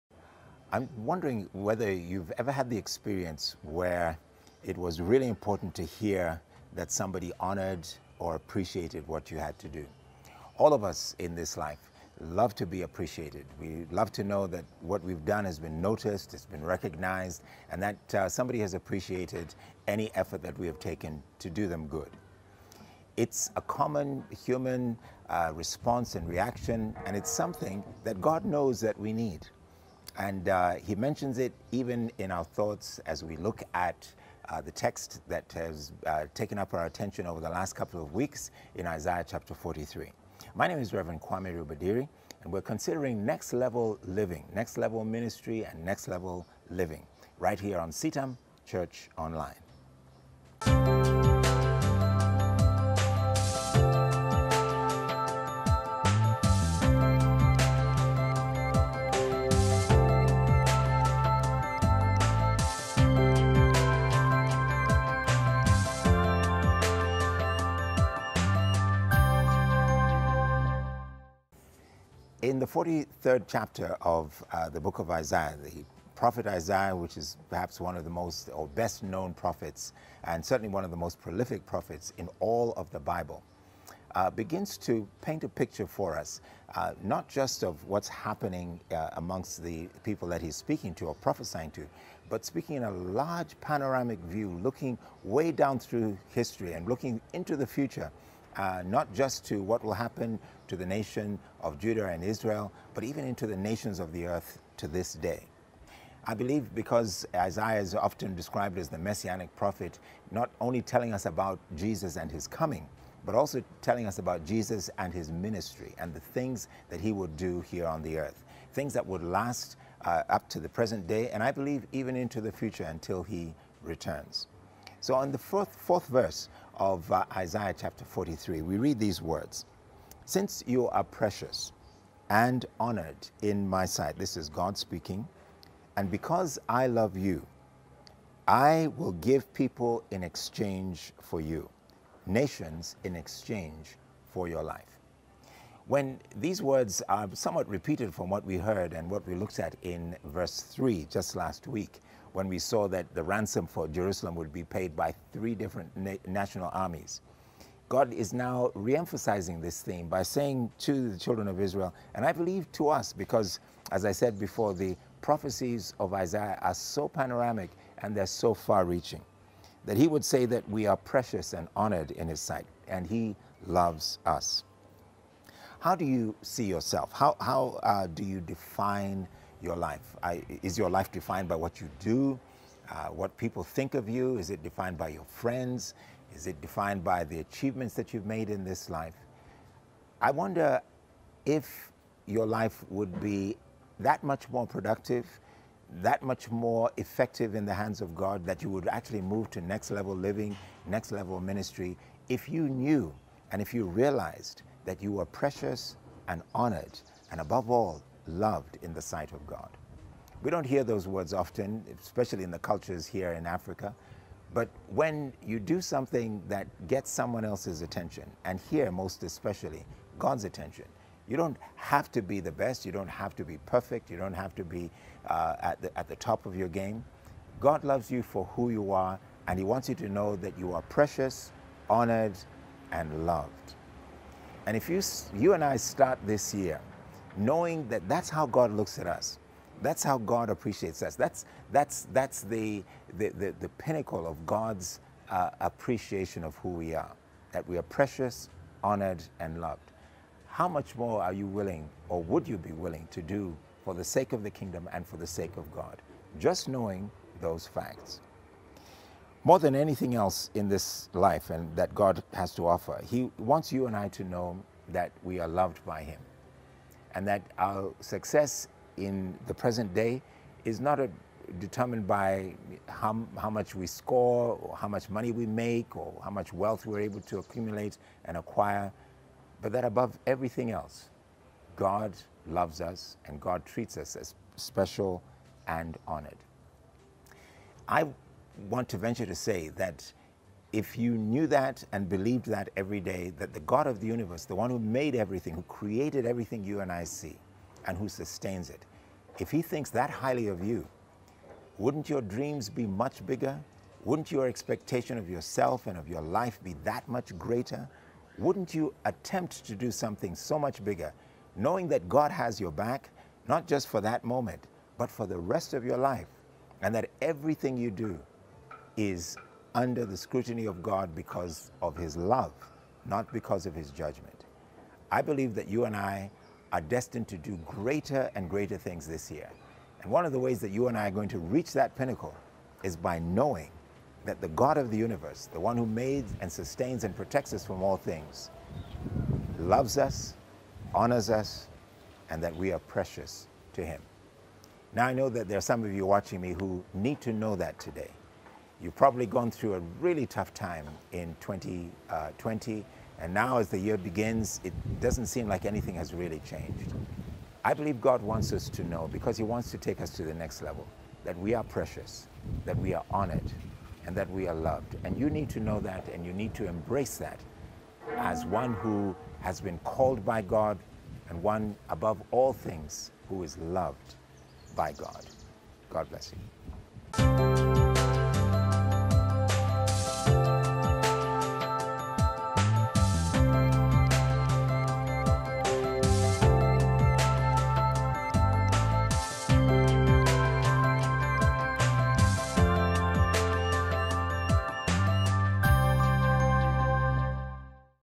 Next Level Living - Rev. Kwame Rubadiri | CITAM Church Online
God thinks very highly of us and if we would believe and live in accordance with how God thinks of us, our dreams and expectations would become much bigger.

Join Rev. Kwame Rubadiri as he teaches us on how our perspective affects our productivity from the topic 'Next Level Living'.